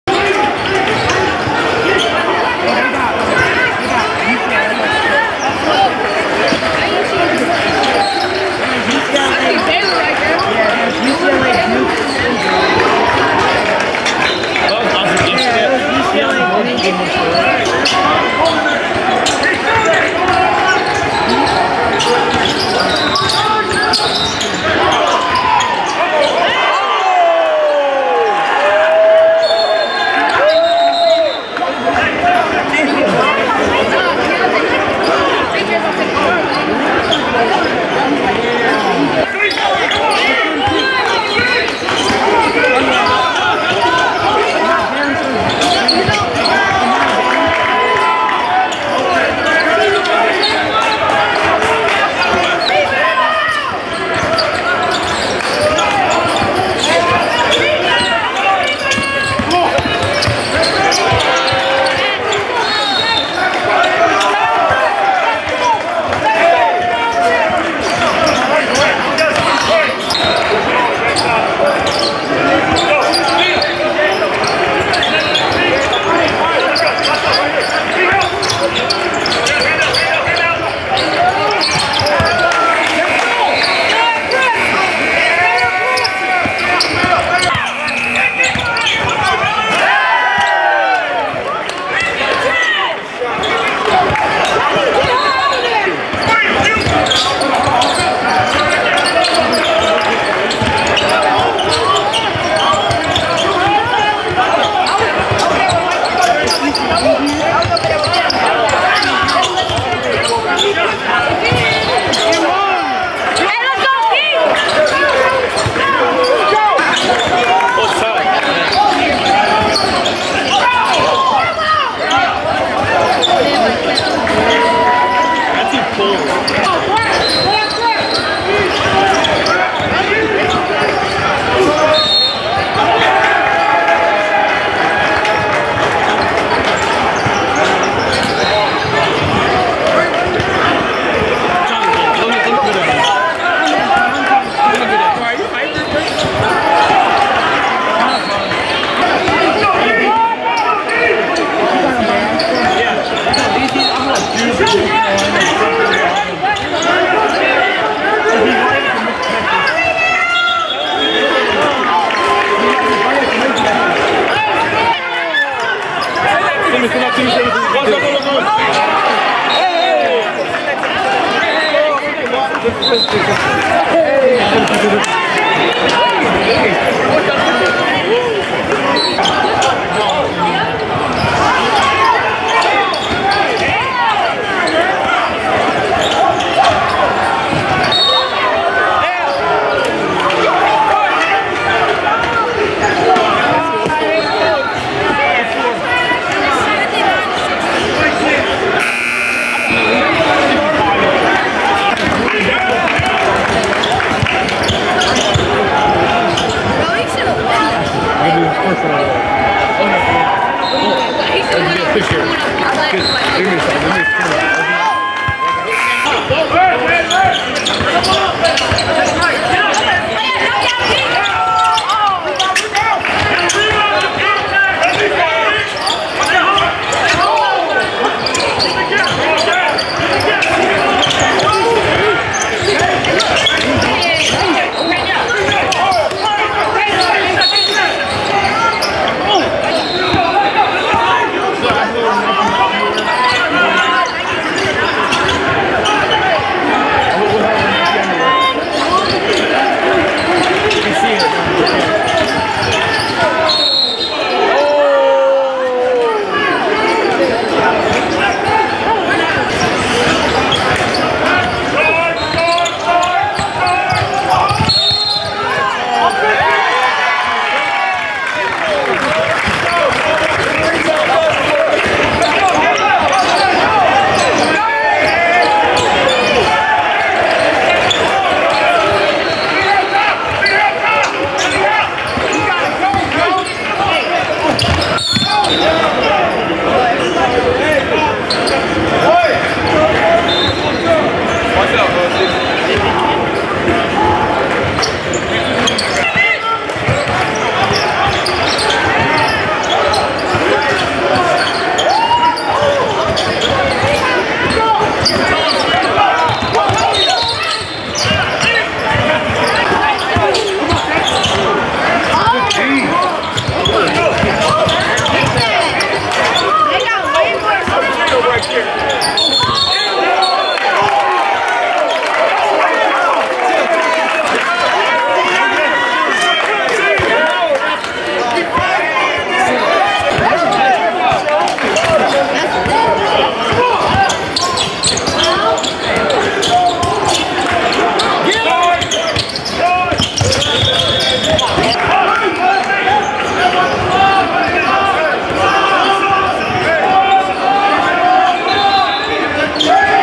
Kansas Commit MJ RICE! (Highlights vs Keyonte George's Team)
6'5" Class of 2022 McDonald's All-American MJ Rice